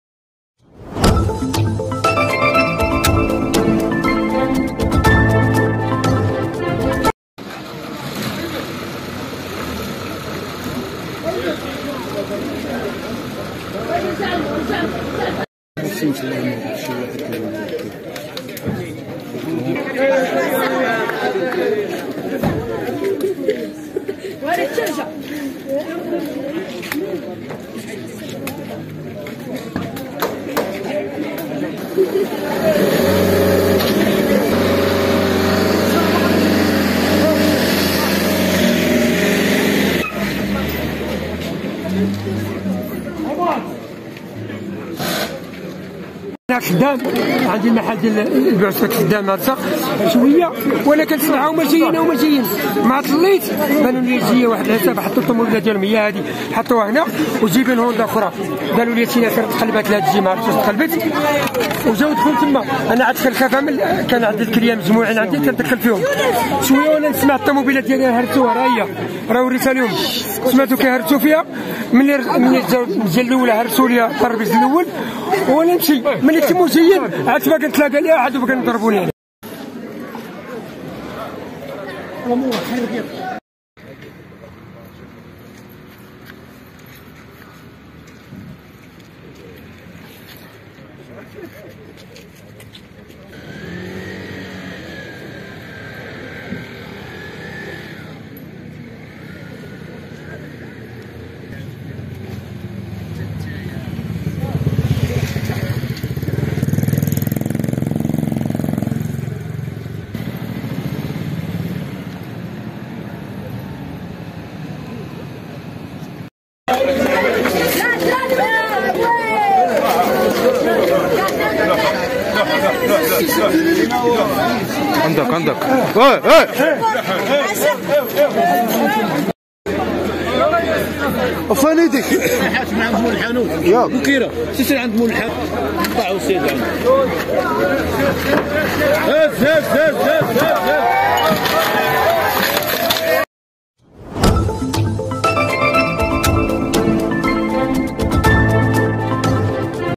حرب عصابات تتسبب في انقلاب سيارتين بمجموعة 4 في حي مولاي رشيد
# أخبار_عاجلة